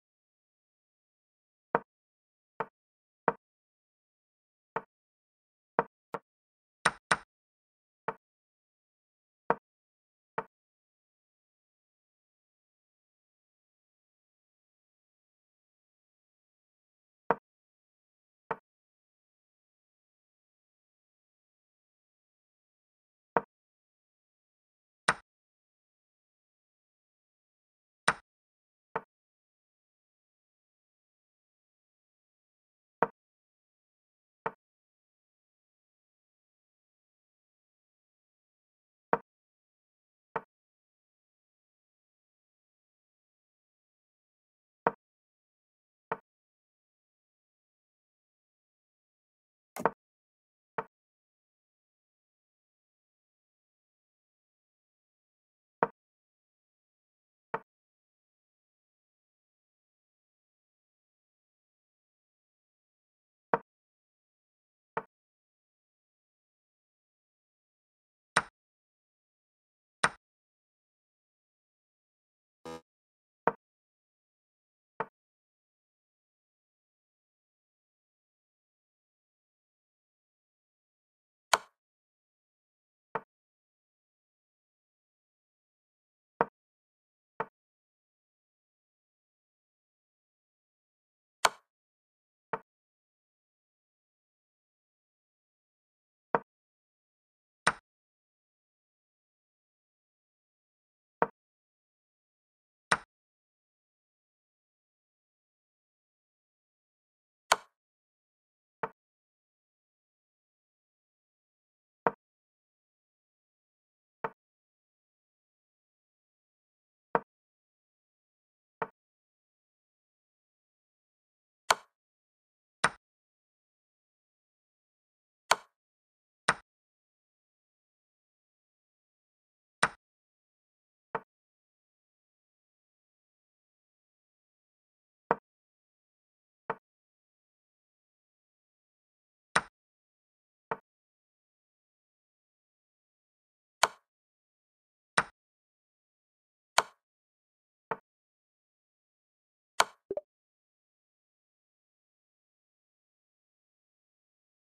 Defeating Engine Level 16!
Defeating all chess bots until I beat all! Today,we have Engine Level16!